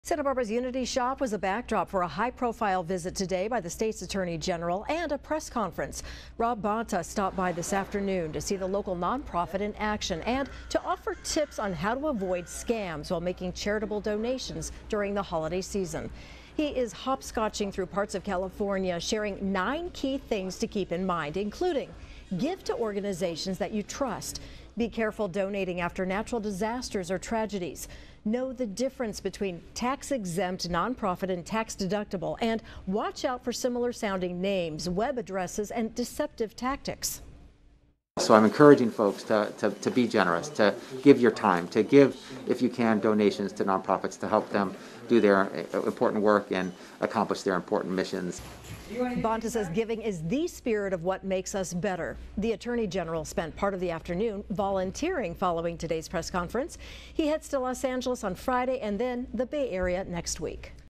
State Attorney General Rob Bonta travels to Santa Barbara to offer holiday consumer tips and ...
State Attorney General Rob Bonta travels to Santa Barbara to offer holiday consumer tips and warnings